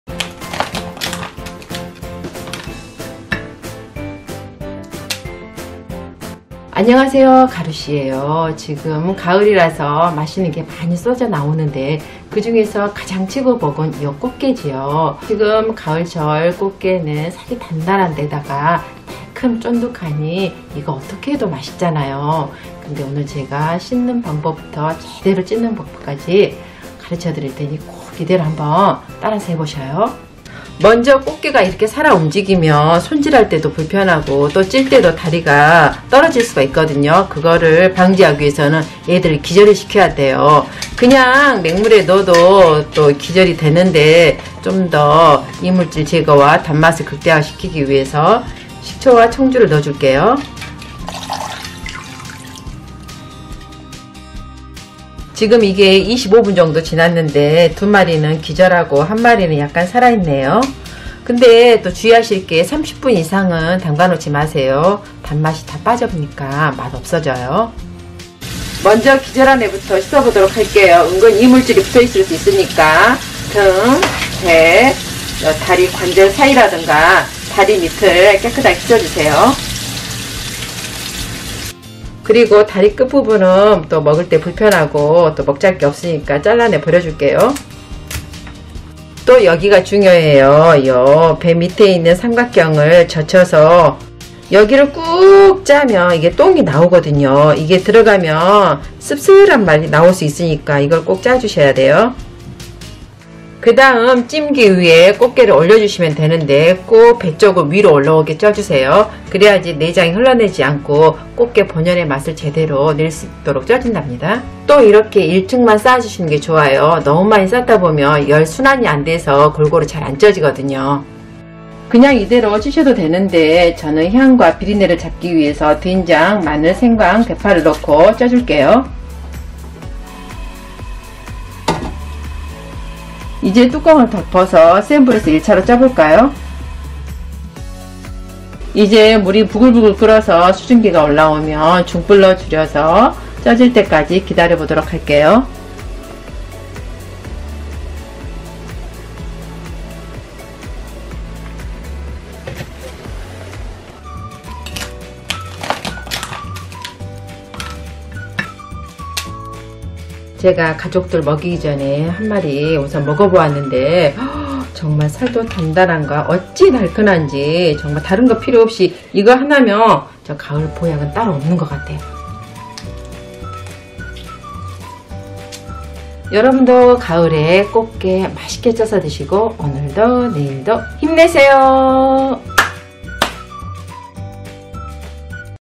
감탄사 절로 나오는 꽃게 맛있게 찌는법. 꽃게 제대로 손질하는법부터 비린내 잡는법까지 자세히!!
가족들 한줄평
엄마: 가격대고 조금 비싸기 했지만,,
그래도 너무 맛있게 잘 먹어서 대만족!
아빠: 살이 먹잘 게 많고, 맛있음.
딸: 살이 고소하고, 커서 빼먹기 편함.
총평: 가을에 안 먹고 지나가면
1년은 섭섭할 것 같은 최고의 맛ㅎㅎ

[살아있는 꽃게 손질법]
1. 먼저 살아있는 꽃게 3마리(1.5kg)를
이렇게 넉넉한 물에 담가주세요.
그리고 여기에 식초 1T와 청주 1.5T을 함께 넣어주면
비린내와 뻘도 없애고, 단맛도 더해준답니다.
이제 20~30분 동안 놔둬 기다려주면 OK.
(저는 커서 30분 정도 걸리더라고요.)

2. 그리고 물을 묻힌 후, 등, 배, 다리 밑 등등..
사이사이를 칫솔로 문질러가면서
깨끗하게 씻어주면 된답니다.

3. 그다음 다리 끝부분은 살이 없기도 하고,
나중에 먹을 때 걸리적거리니깐.
이렇게 가위로 잘라내 버려주세요.

4. 요 배쪽 삼각형을 밑으로 내리고,
손으로 밑에서 위로 올리면서 짜주면
이렇게 거무튀튀한 배설물이 나올 거예요.
이게 들어가면 나중에 맛이 텁텁하니깐,
이렇게 미리미리 짜내 버려주는 게 좋겠죠

[꽃게찌는법]
1. 이제 찜기에 올려 찌시면 되는데.
이때 주의할 게. 반드시 배가 위로 가도록!
그래야 내장이 빠져나오지 않고,
깔끔하니 나중에 맛있게 먹을 수 있답니다.

2. 또 그냥 물에 올려 찌셔도 괜찮지만.
저는 꽃게 비린내도 잡고 향을 더하기 위해,
대파 1/2대, 얇게 썬 통마늘 3알,
편생강 두쪽, 된장 1/2T을 넣어주었답니다.
이건 필수는 아니니 취향껏 추가로ㅎㅎ

3. 이제 뚜껑을 덮은 다음, 먼저 센불에서 1차로 한번 쪄주세요.
그리고 물이 끓어서 수증기가 나오면,
이제 중불로 줄여 15분 동안 쪄주세요.
*제건 크기가 커서 이정도 찌면 딱! 맞던데.
작으면 12~13분 정도로 될 듯.
(그래도 잘 살펴봐주는 게 좋겠죠ㅎㅎ)

어때요? 붉은 색감만 봐도 정말 맛나 보이죠!
제거는 좀 사이즈가 크고 싱싱하다 보니
가격대가 평소 사 먹는 것보다 비싸긴 한데,
확실히 그만큼 안에 살도 꽉! 차있어서.
진짜 먹을 맛이 제대로 난다고나 할까ㅎㅎ

그러면서도 비린 맛없이 깔끔고소해서,
진짜 다른 반찬 없이 이것 하나만 있어도
밥이 술술~ 맛있게 넘어가더라고요♥
진짜 꽃게를 너무 맛있게 잘 먹어서
가을에 안 먹고 지나가면 섭섭하달까!!

여러분도 가을에는 살이 꽉 차있는
싱싱한 꽃게 맛있게 쪄드시고
오늘도 내일도 모두 모두 힘내시길 바랍니다!